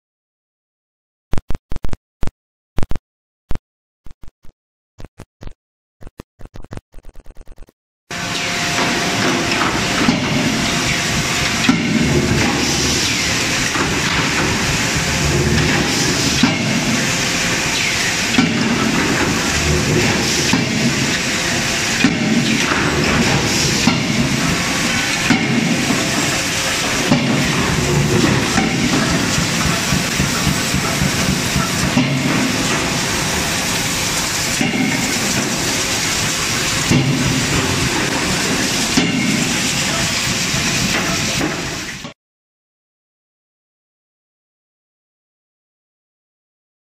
Aviation Snips Forging Hammer, Drop Forging Hammer Line
This forging line is designed to make aviation snips, the line machine include heating machine, Anyang 16kj CNC drop forging hammer, trimming press, etc.